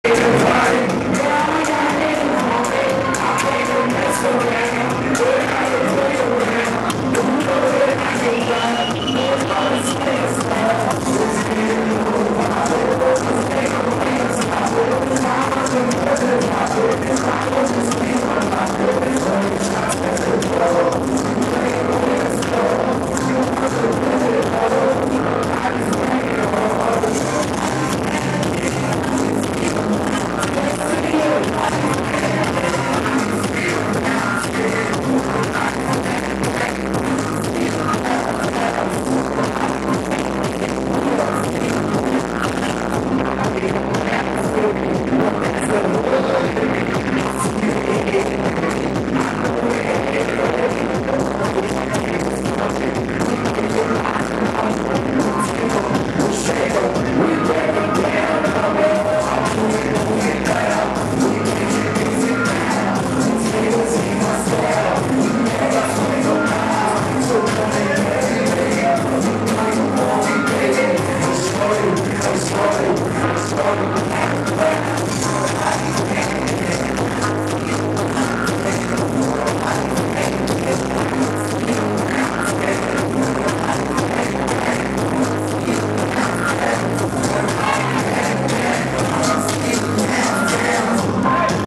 Winnipeg's Most Live in Saskatoon with DJ Sugar Daddy
Bang Bang - Special Live Concert at the Pat with DJ Sugar Daddy - Check the pics on facebook
click LIKE on the page to tag your friends! Follow if you have Twitter @DjSugarDaddy306